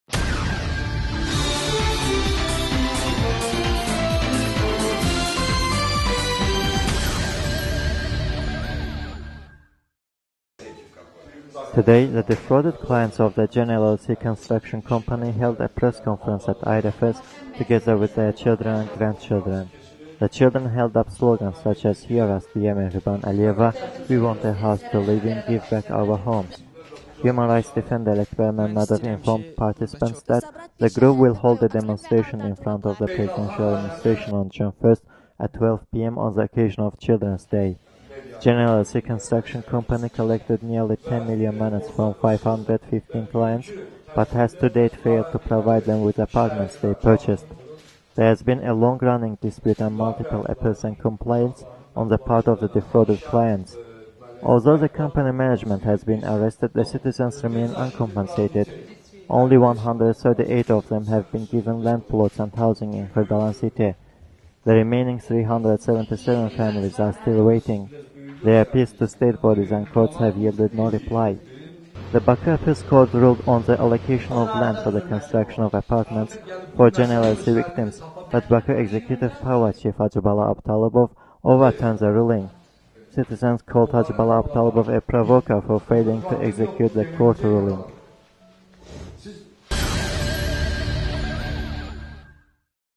CHILDREN TO ADVOCATE FOR FRAUD VICTIMS
Today the defrauded clients of the Gen LLC construction company held a press conference at IRFS together with their children and grandchildren.

The children held up slogans such as "Hear us, dear Mehriban Aliyeva!", "We want a house to live in", "Give back our homes!". Human rights defender Alakbar Mammadov informed participants that the group will hold a demonstration in front of the Presidential Administration on June 1st at 12:00pm, on the occasion of Children's Day.

Gen LLC Construction Company collected nearly 10 million manats from 515 clients, but has to date failed to provide them with the apartments they purchased. There has been a long-running dispute and multiple appeals and complaints on the part of the defrauded clients. Although the company management has been arrested, the citizens remain uncompensated. Only 138 of them have been given land plots and housing in Khirdalan city. The remaining 377 families are still waiting. Their appeals to state bodies and courts have yielded no reply.

The Baku Appeals Court ruled on the allocation of land for the construction of apartments for Gen LLC victims, but the Baku Executive Power chief Hajibala Abutalibov overturned the ruling. Citizens called Hajibala Abutalibov a  "provoker" for failing to execute the court ruling.